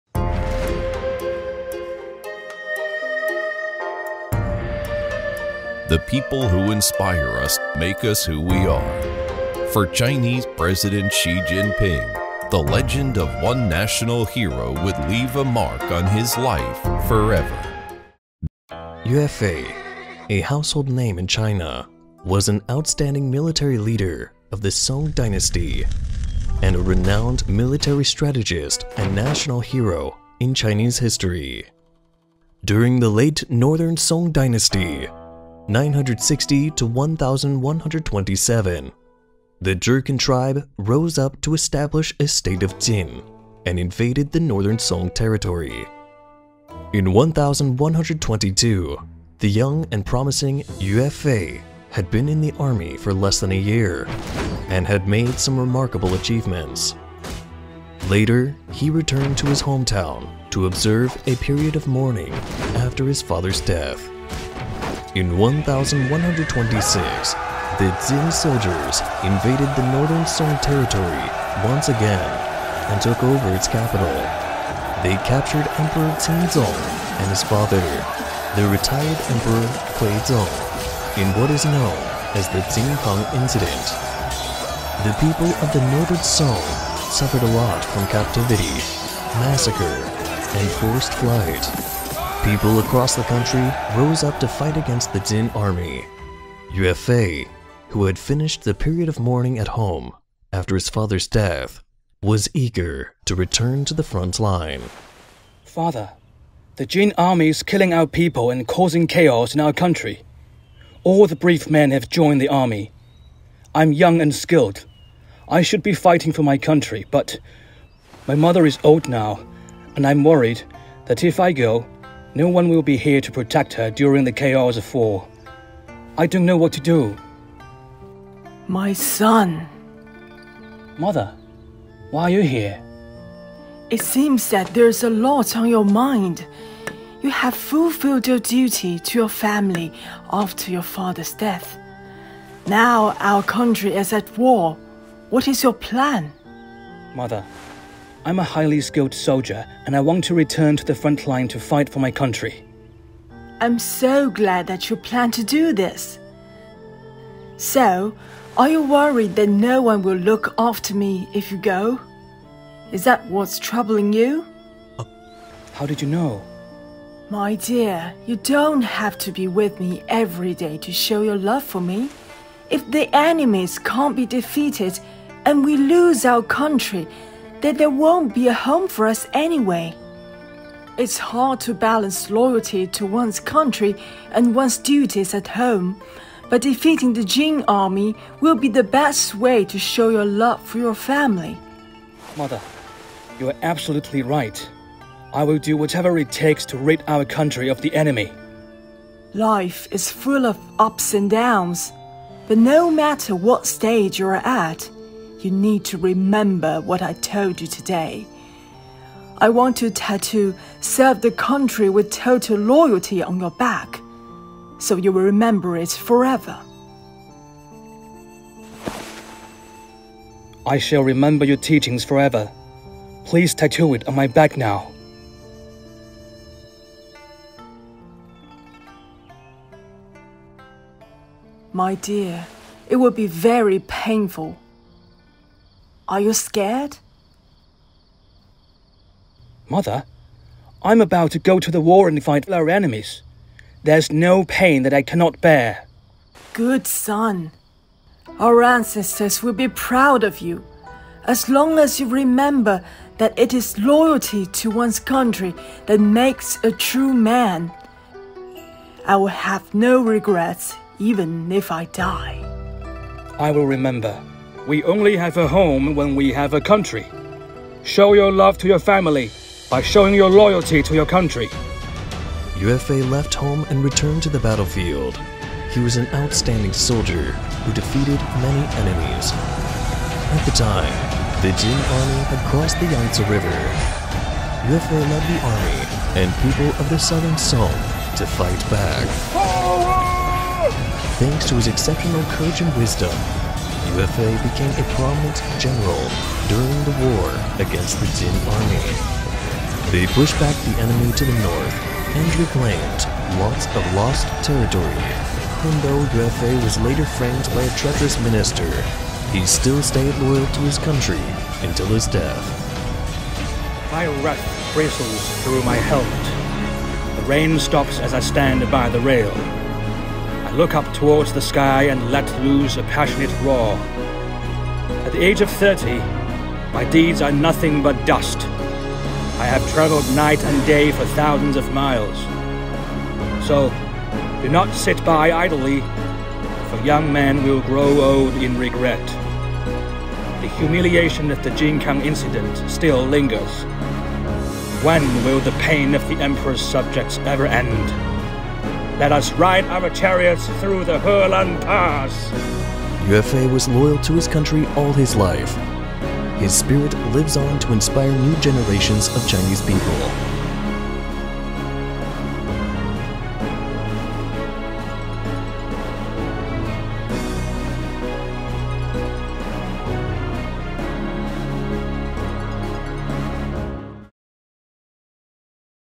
Cartoon | Serving the nation with total loyalty
The people who inspire us make us who we are. For Chinese President Xi Jinping, the legend of one national hero would leave a mark on his life forever.